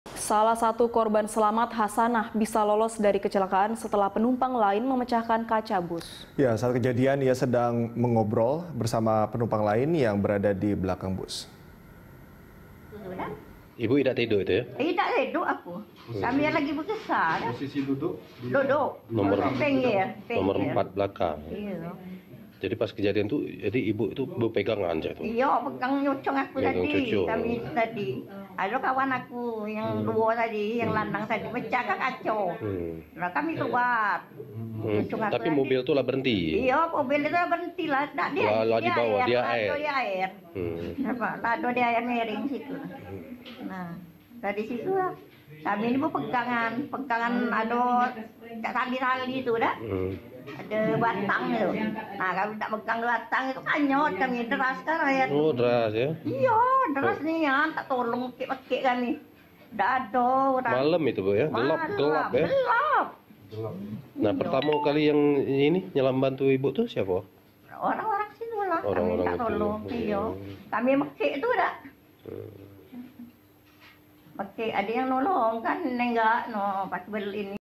Seorang Penumpang Beberkan Lolos dari Kecelakaan Bus Maut
Seorang korban selamat bus maut di Pagar Alam, Sumsel, bernama Hasanah menuturkan bisa lolos dari kecelakaan setelah penumpang lain memecahkan kaca bus. Saat kejadian ia sedang mengobrol bersama penumpang lain di belakang bus.

Facebook.com/BeritaSatu
Youtube.com/BeritaSatu